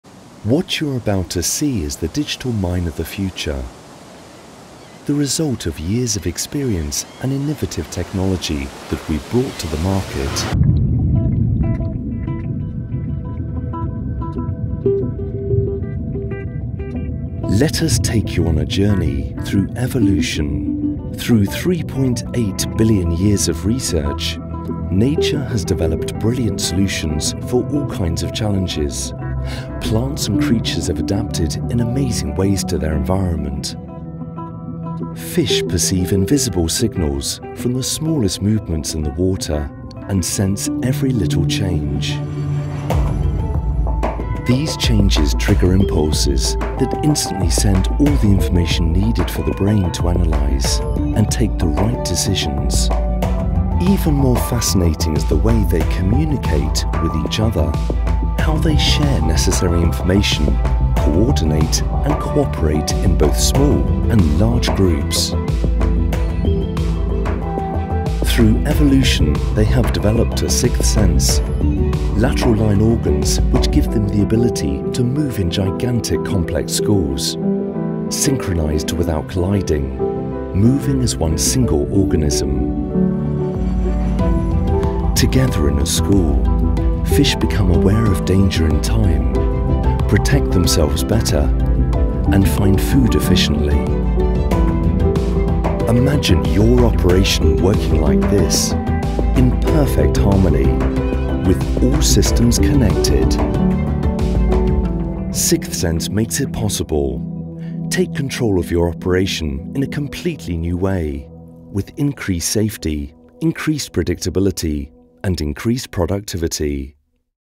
Explainer video for Atlas Copco – '6th sense'
A film we made in full CGI, that describes Atlas Copco artificial intelligence products by using nature as an example of motion detection and more. Everything is made in CGI, and we used 3D Studio Max and Octane Render.

Client: Atlas Copco
Agency: Strateg
Director: FILMIC ART
Production company: FILMIC ART